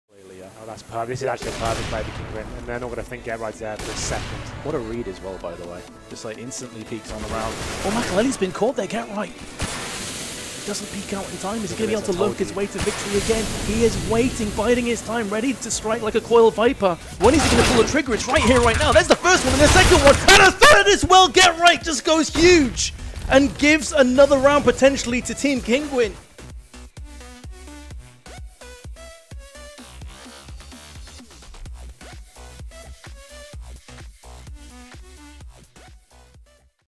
[Frag Clip] GeT_RiGhT's Crazy 3K Lurk
GeT_RiGhT plays the round perfectly and ends up with three kills in this fast clip while subbing in for Kinguin.

Editing Gear:
Video Card: R9 295X2
SSD: Crucial 500GB
CPU Cooler: Be Quite Dark
CPU: Intel i5 4690K
Motherboard: GA-Z97X-SLI

Casting //
Vince "Metuz" Hill and Henry "HenryG" Greer
Thanks to PGL